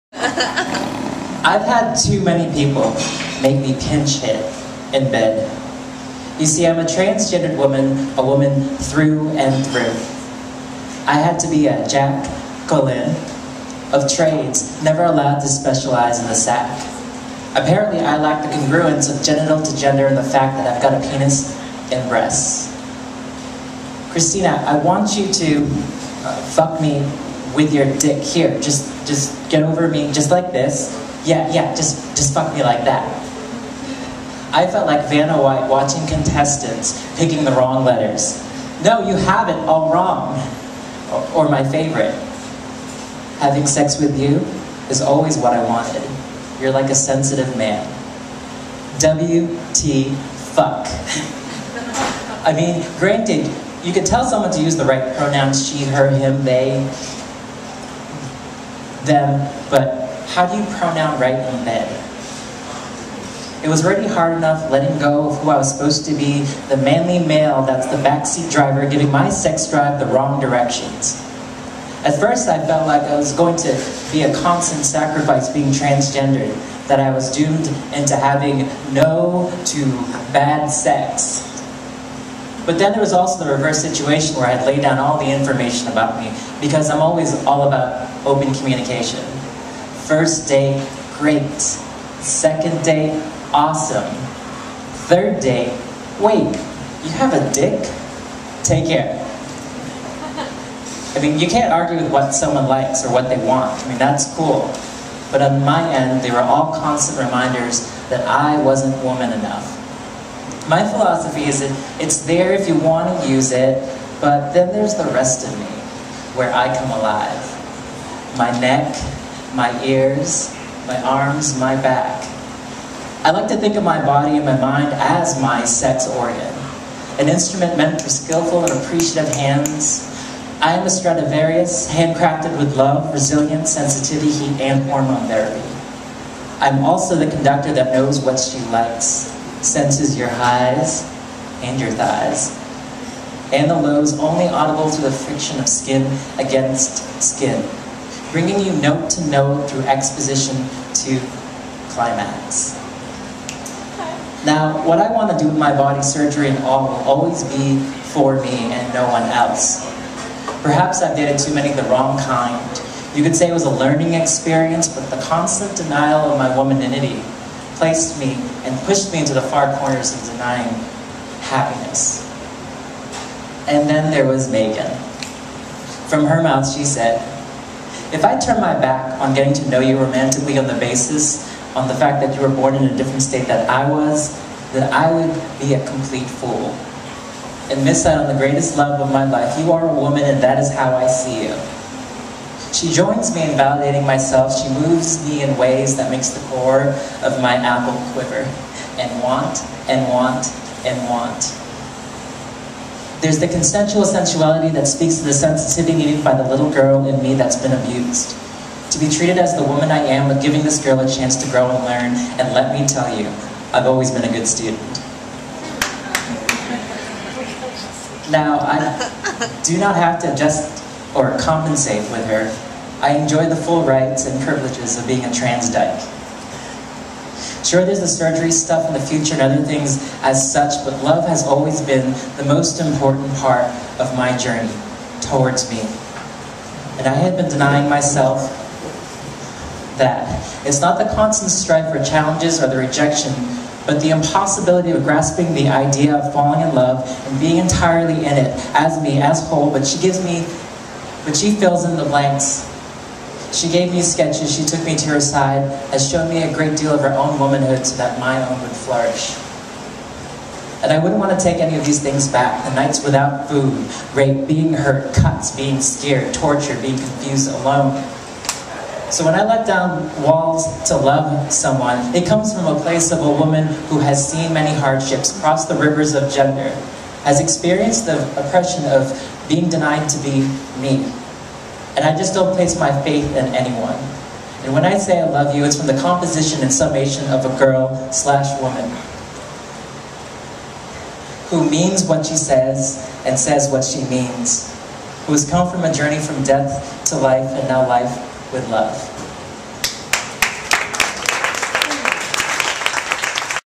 Christine m4v
This transgender female, Christine, gives us some insight into her lifestyle in this long narrative poem, which she read at Club Hairspray in Asheville, NC in 2011.